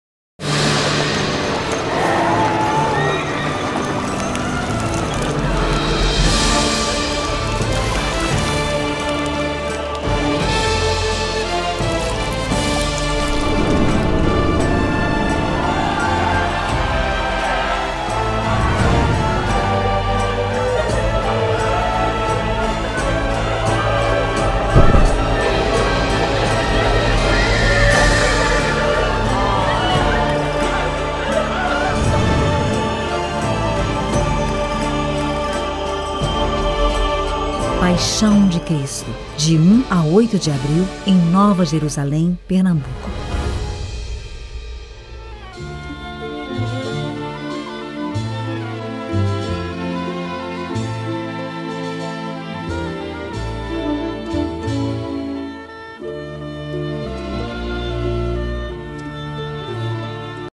PAIXÃO DE CRISTO DE NOVA JERUSALÉM 2023 - FILME 01
Primeiro filme da Campanha Publicitária da Temporada 2023 da Paixão de Cristo de Nova Jerusalém.
Roteiro e Direção: Eduardo Morotó
Diretor de Fotografia: Beto Martins
Diretora de Arte: Helga Queiroz
Figurino: Marina Pacheco
Produtora: Virtual Filmes
Coordenador Geral pela STFN: Luciano Telles
Realização: Sociedade Teatral de Fazenda Nova - STFN